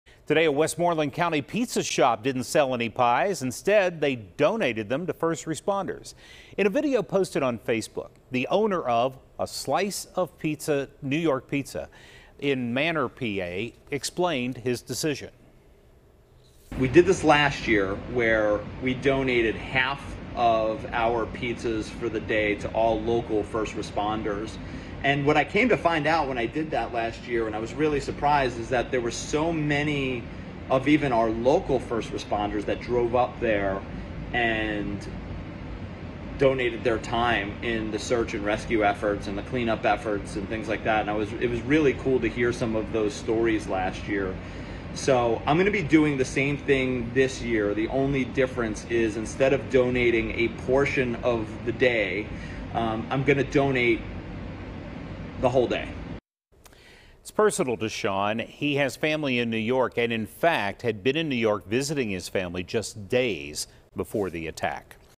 Slice Of New York Pizza Donates Pizza To First Responders
A Slice of New York Pizza in Manor was "closed" on Saturday, donating all pizza to local first responders.